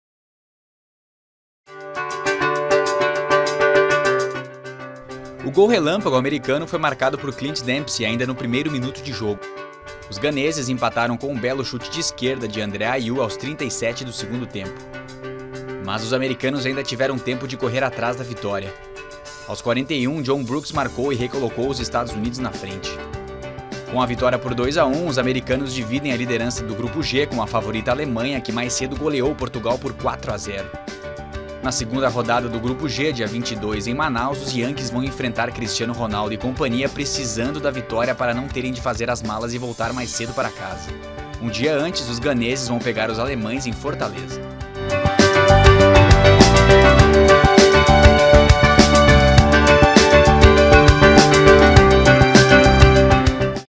EUA fazem gol relâmpago e vencem Gana por 2 a 1
Dia do primeiro jogo sem gols, entre Irã e Nigéria, mas também do gol mais rápido: os americanos estrearam na Copa contra Gana e marcaram logo aos 28 segundos.